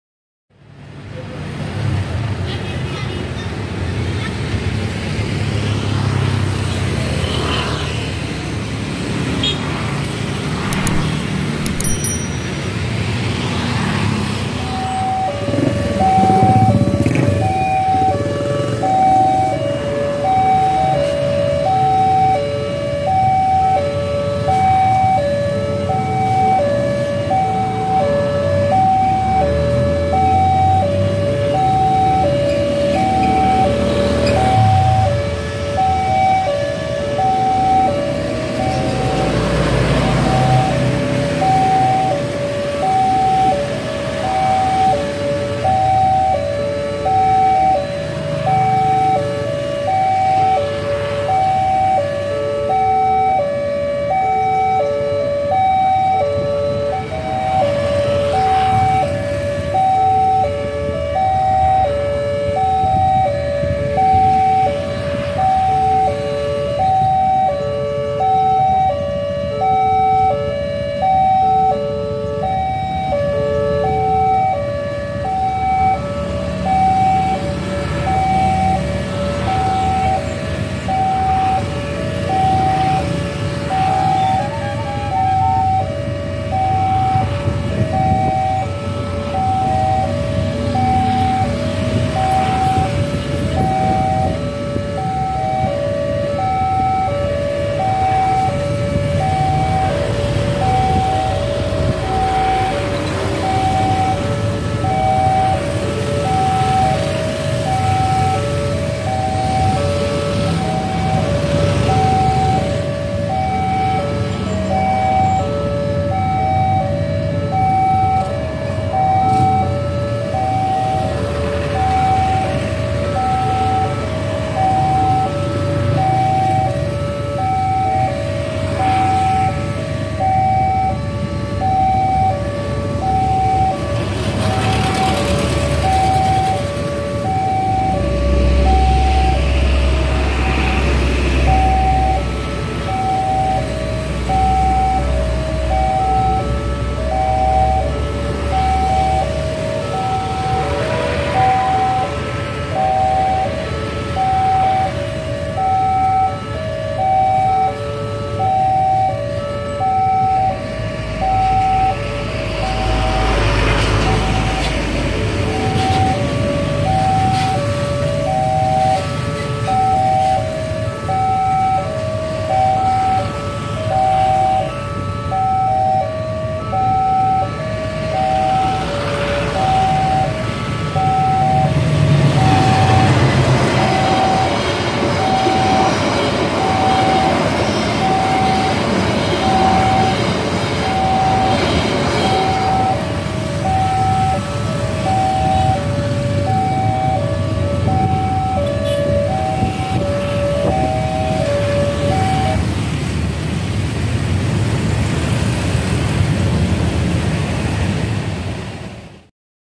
Ka Argo Bromo Anggrek melintas di PALANG PINTU KERETA API JPL 278 Brebes // Railroad Crossing
Perlintasan Palang Pintu Kereta Api JPL 278 Brebes melintas Ka Argo Bromo Anggrek